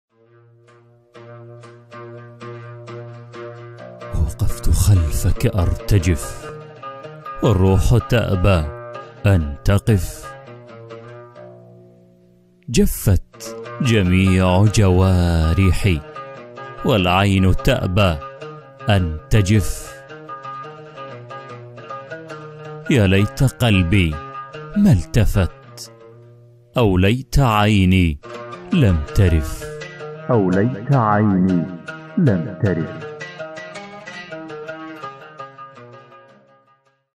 وقفت خلفك أرتجف .. والروح تأبى أن تقف ¦ ¦ من جميل ما قيل في المحبة
كلمات الشاعر عادل الحصيني

للصوت أناقة.. الصوت المُتَأنّق